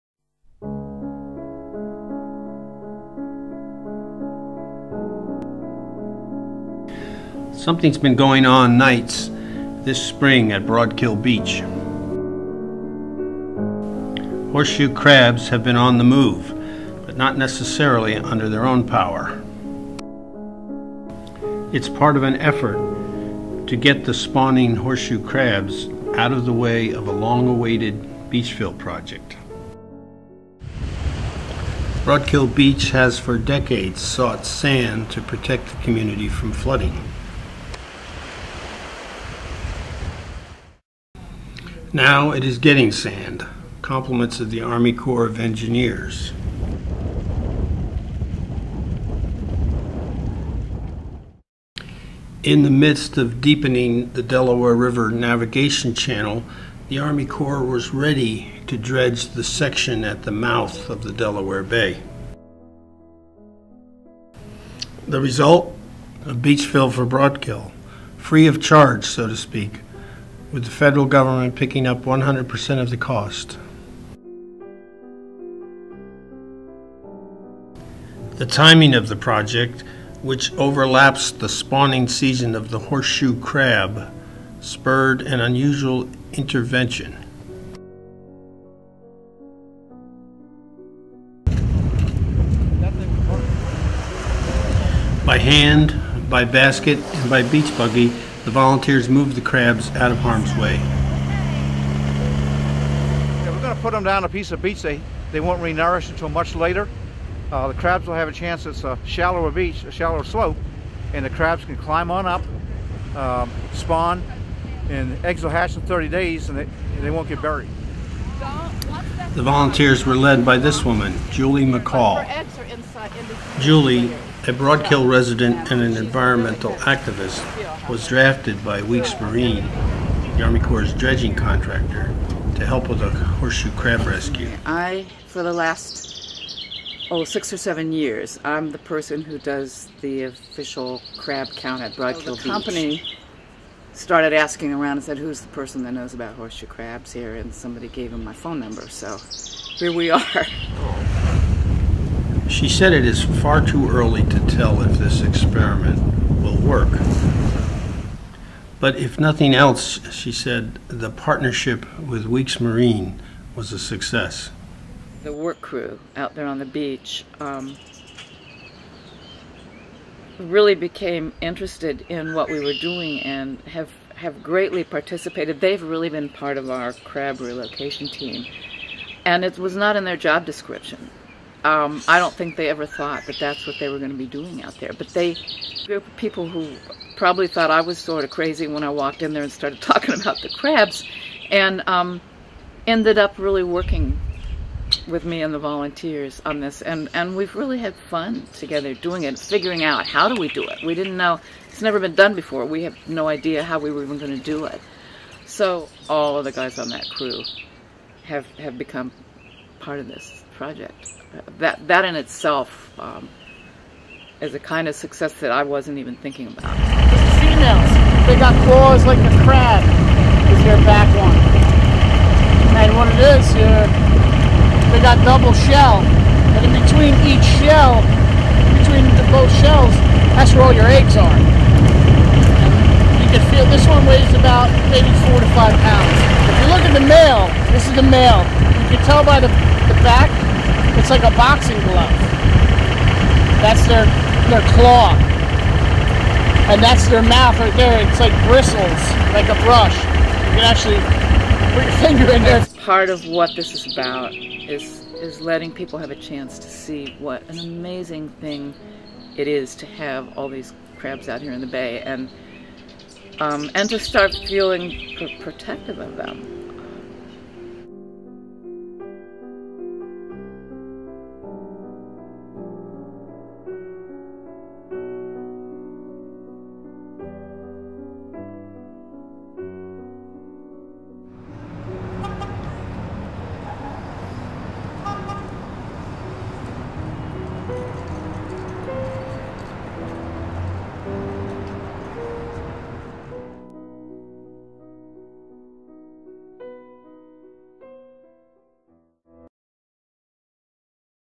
Moving horseshoe crabs out of the way
Army Corps of Engineers, Weeks Marine & volunteers move spawning horseshoe crabs out of the way of a beachfill project on Broadkill Beach, Delaware